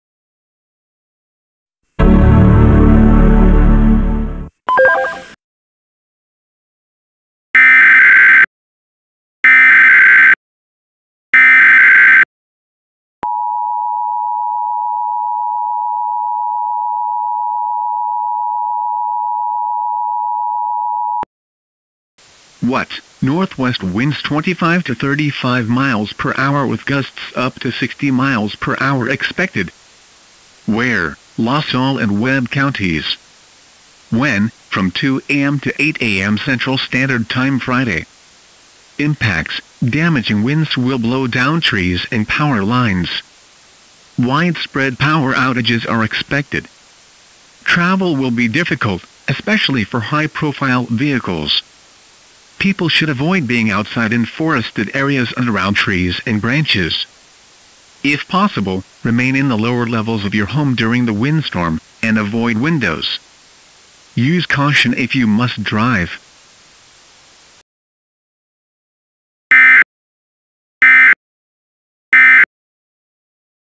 EAS Reenactment Mock: High Wind Warning for Webb and LaSalle counties - 01/11/2024
Yep, I am already making a reenactment on that High Wind Warning Corpus just issued for Webb and La Salle counties. It's been a while since I have done a reenactment mock.

URGENT - WEATHER MESSAGE
National Weather Service Corpus Christi TX
202 PM CST Thu Jan 11 2024

...HIGH WIND WARNING IN EFFECT FROM 2 AM TO 8 AM CST FRIDAY...

* WHAT...Northwest winds 25 to 35 mph with gusts up to 60 mph
  expected.

* WHERE...La Salle and Webb Counties.

* WHEN...From 2 AM to 8 AM CST Friday.

* IMPACTS...Damaging winds will blow down trees and power lines.
  Widespread power outages are expected. Travel will be
  difficult, especially for high profile vehicles.

PRECAUTIONARY/PREPAREDNESS ACTIONS...

People should avoid being outside in forested areas and around
trees and branches. If possible, remain in the lower levels of
your home during the windstorm, and avoid windows. Use caution if
you must drive.